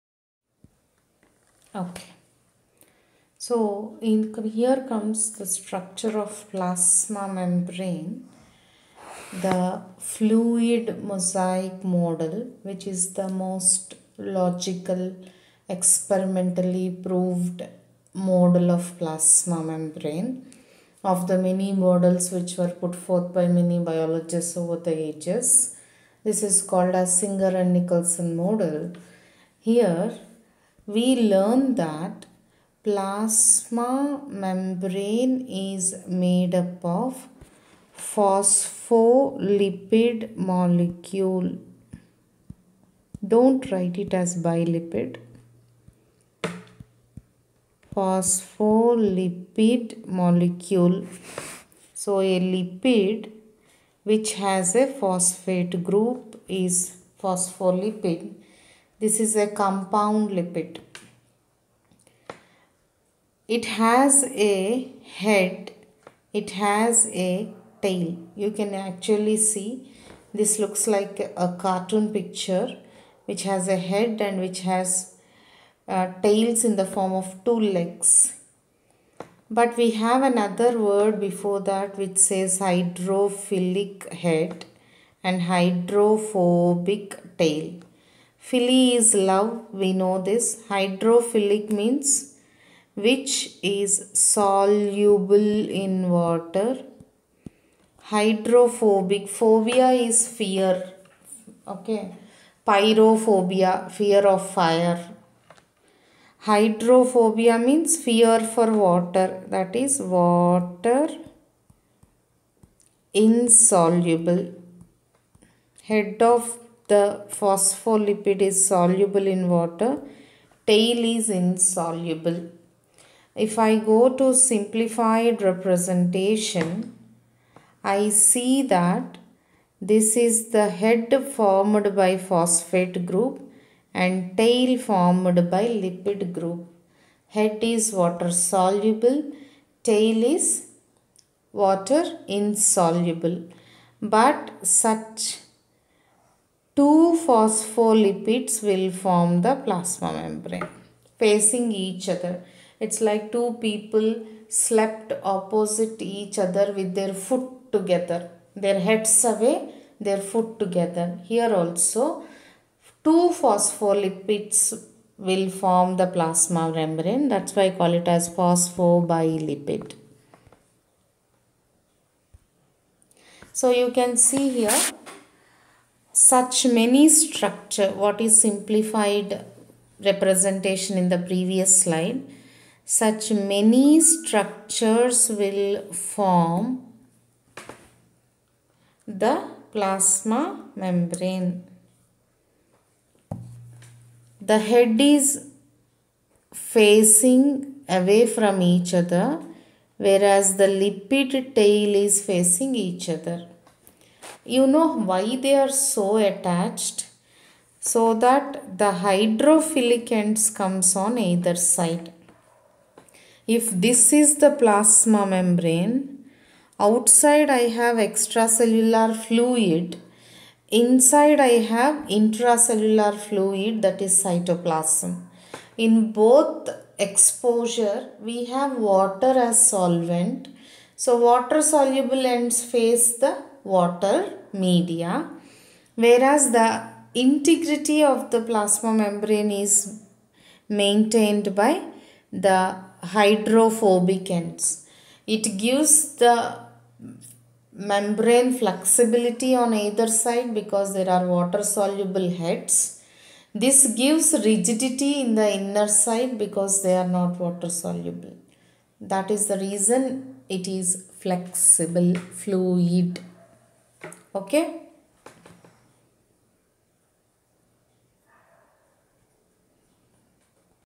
PU 1 | NEET | Biology | Cell: Unit of Life | Lecture 04 | Slide 07 | Phospho Bilipid Layer
Welcome to Concept Videos for NEET! This slide will explain Cell: Unit of Life. We will focus on Phospho Bilipid Layer. We are sure you will have an effective learning experience.